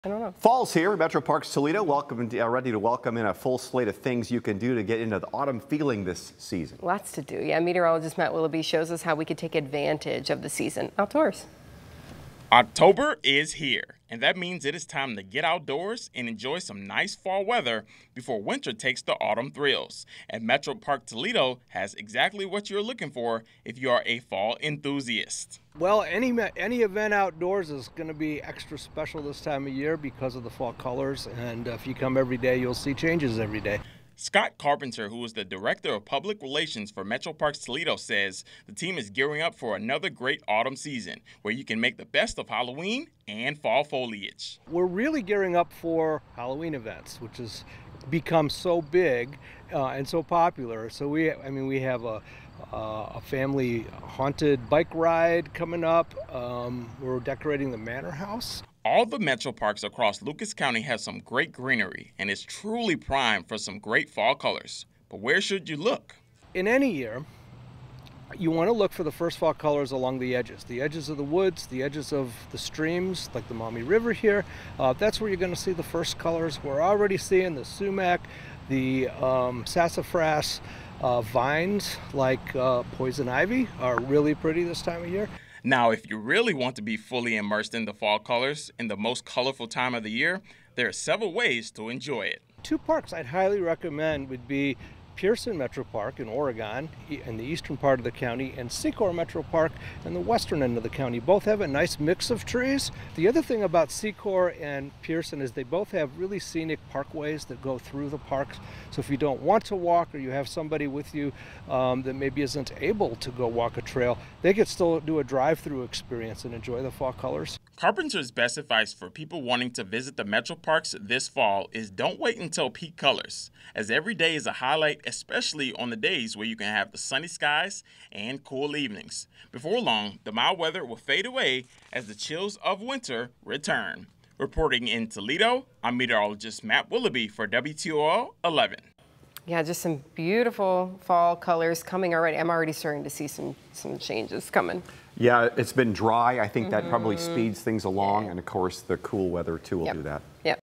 Metroparks Toledo has variety of offerings for fall enthusiasts
Metroparks Toledo's public relations director says the team is gearing up another great autumn season where you can make the best of Halloween and fall foliage.